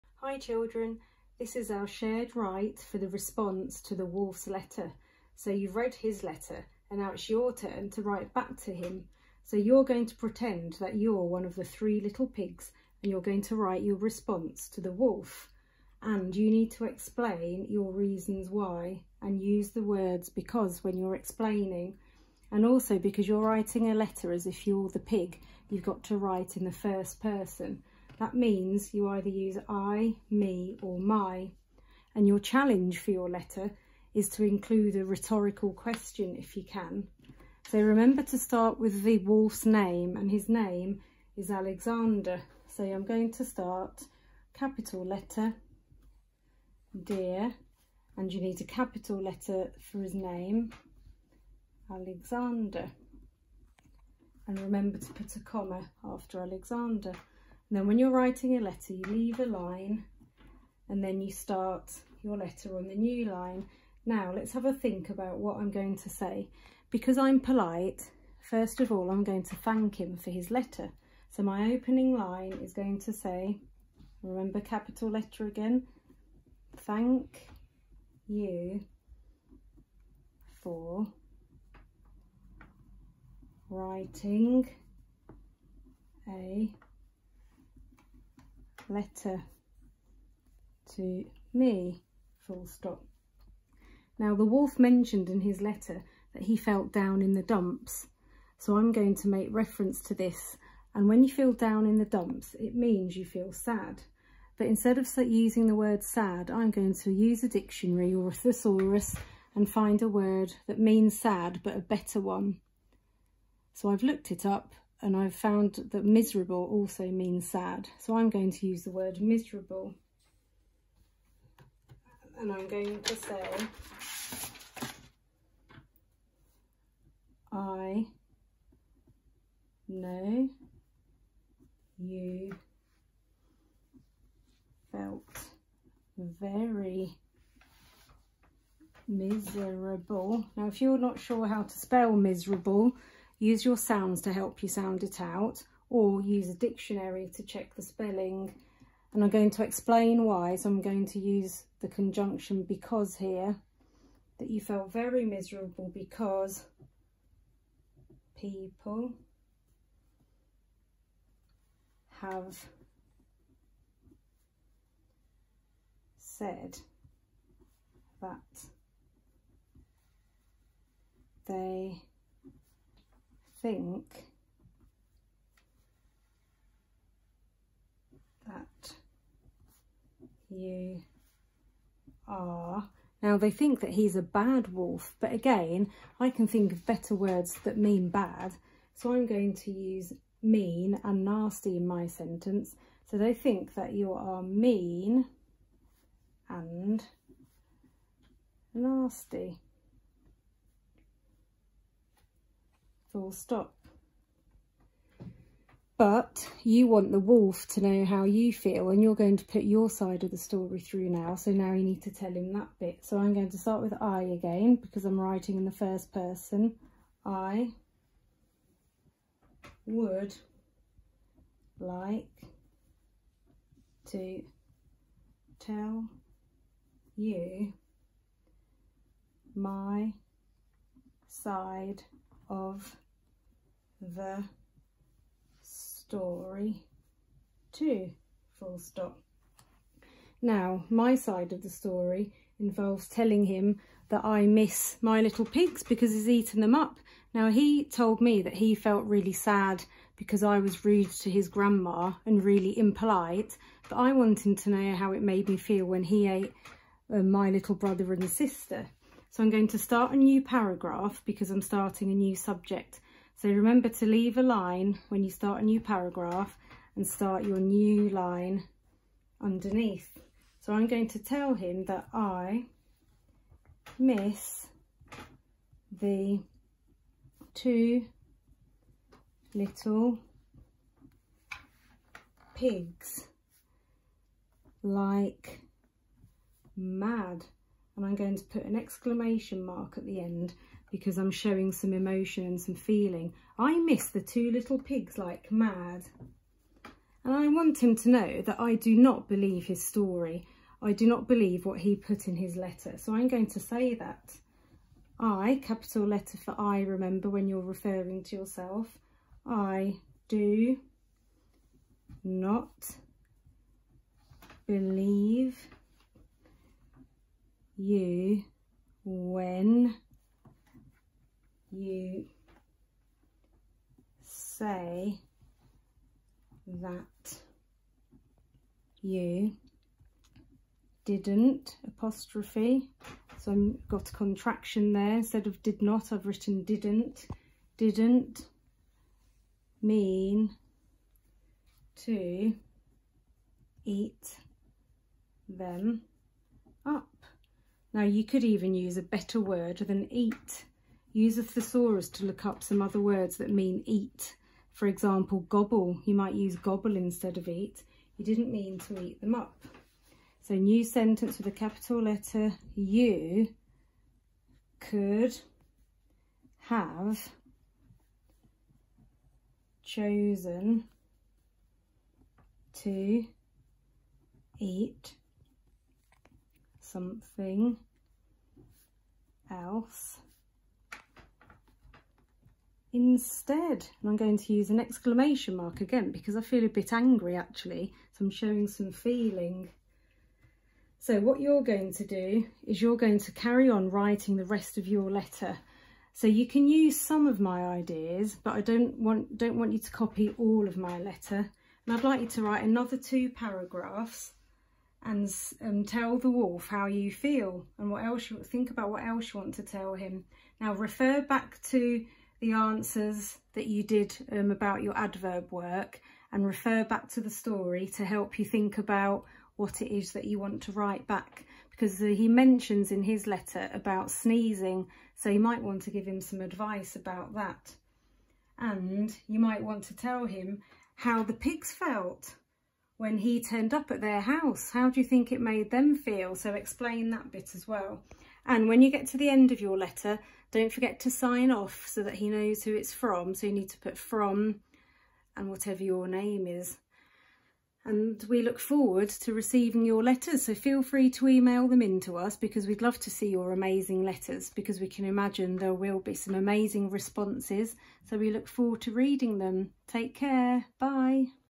Shared Letter Writing -Activity 3
Watch Mrs Pelosi drafting a response to the big, bad wolf!